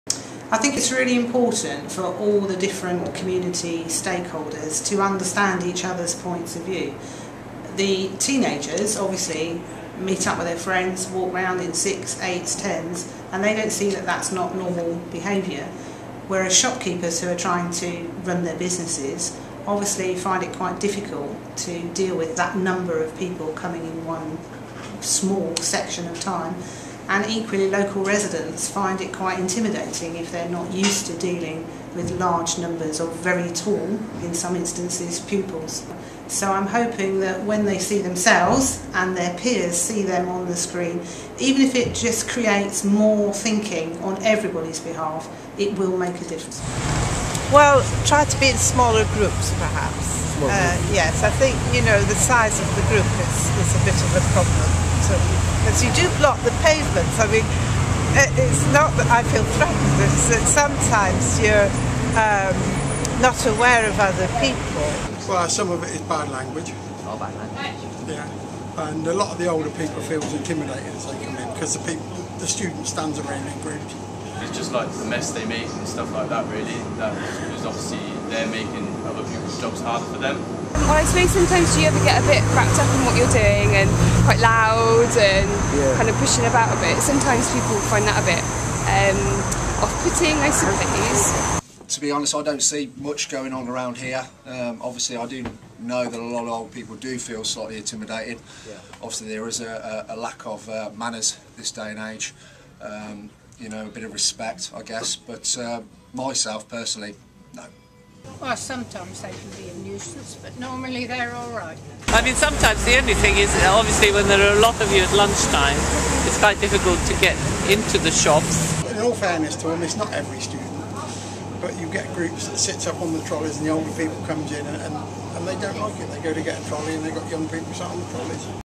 Kids investigate anti-social behaviour - the interviews
Armed with video cameras, local school children interviewed members of their community and local business owners on anti-social behaviour. Assistant Head Teacher at Cherwell School, Julie Stuart-Thompson explains what the project is all about and the impact it will have.

A police perspective on this project can be seen in a second clip.